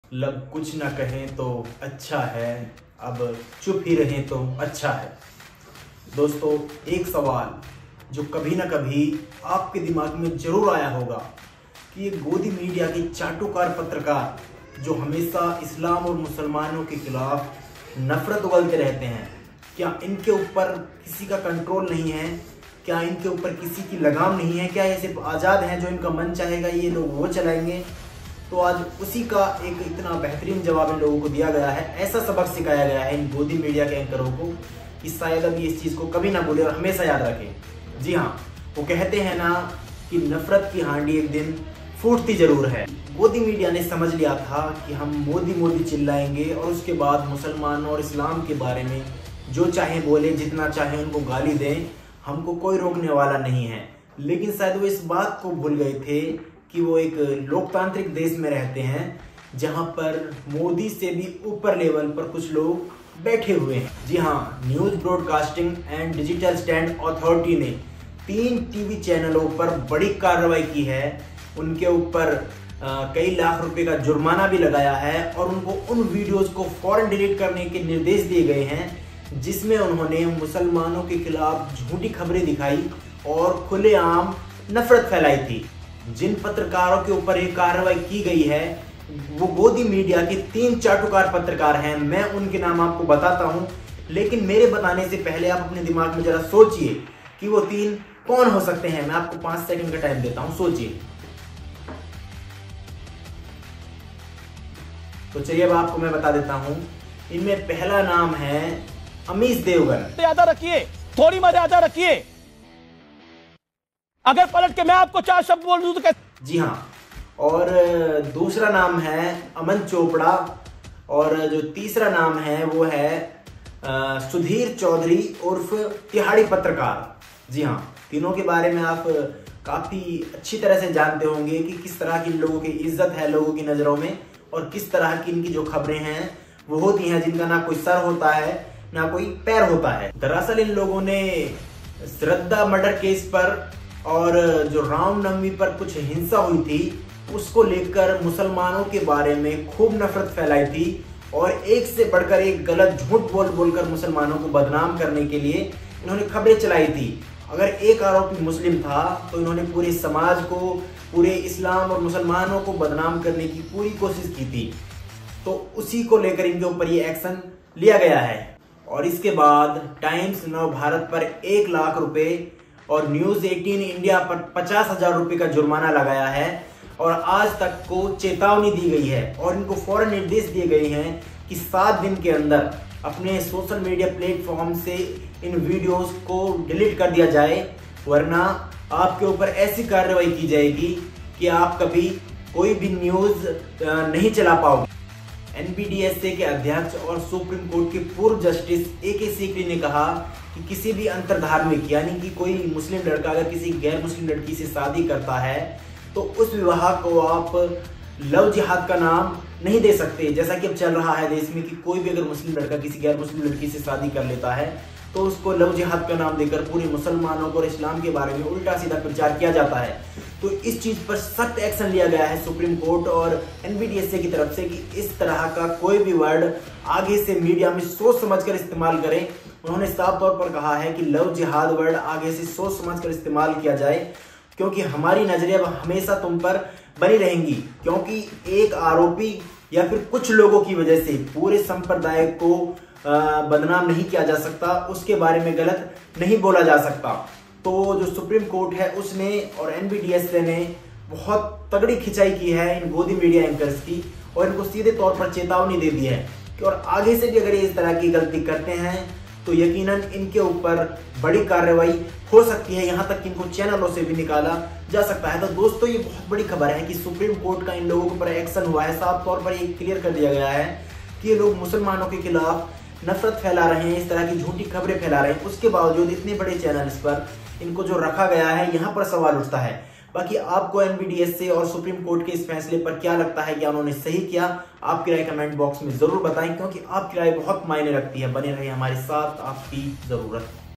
मुसलमानो के खिलाफ चलाई थी झूटी खबरें | Nbdsa action Godi media | Godi Media par Jurmana
Godi media news
Godi media par karyvahi
Godi media action
Muslim news
Amis devgan aman chpda sudhir choudhry